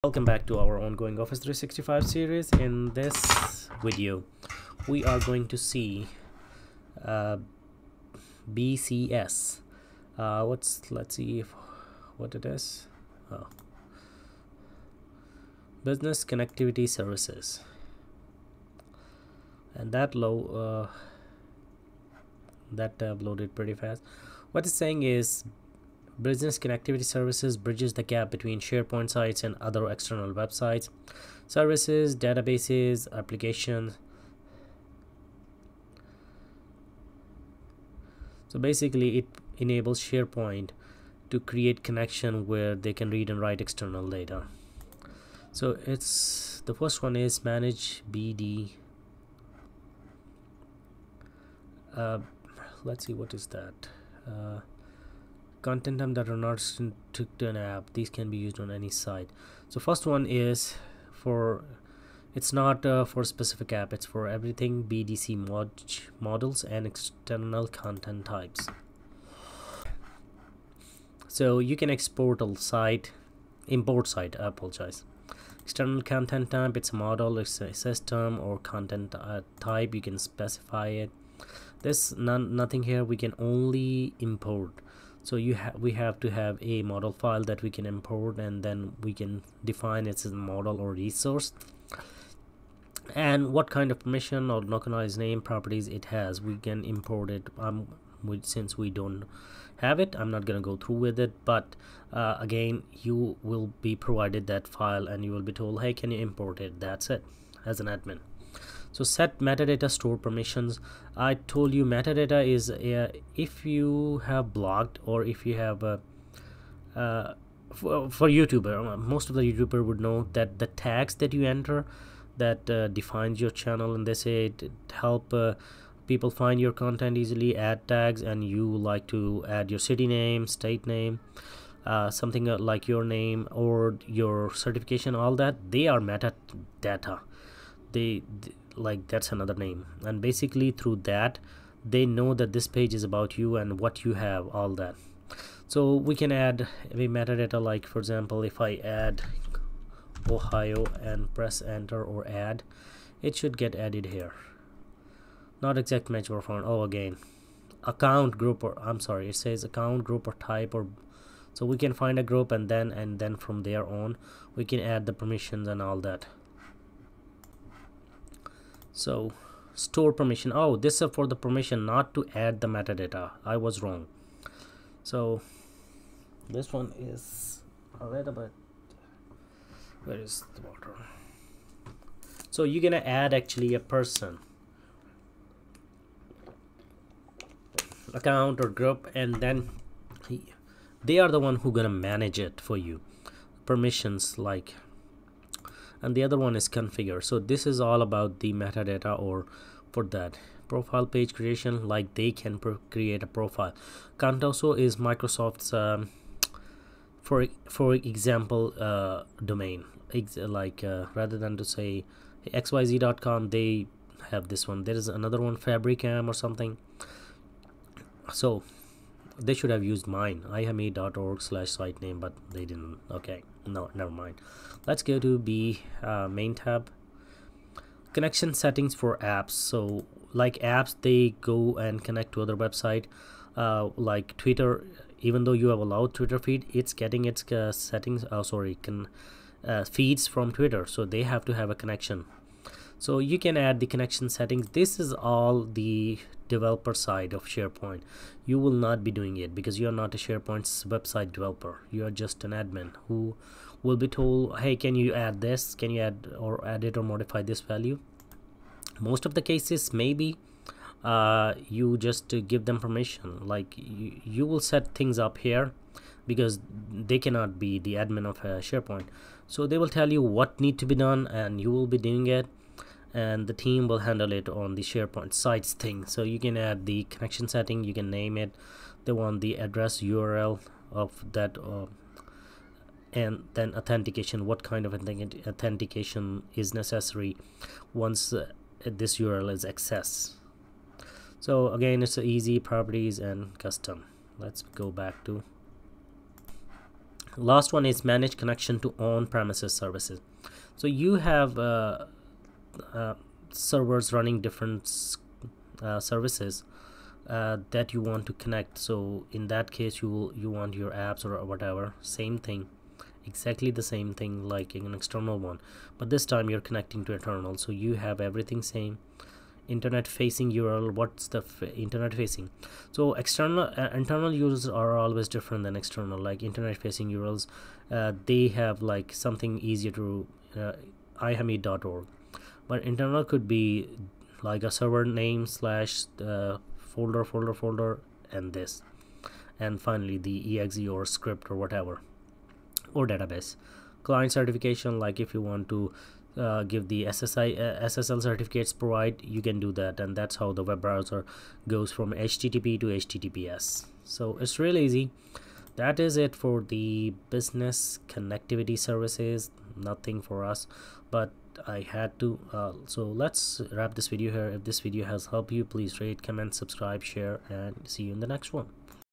OO44 - Business Connectivity Services | SharePoint | Office365
in this video we explore business connectivity center & how to
Manage BD C Models and Externals Content Types
connection settings for apps
Manage Connections to online services
Manage connections to on-premises services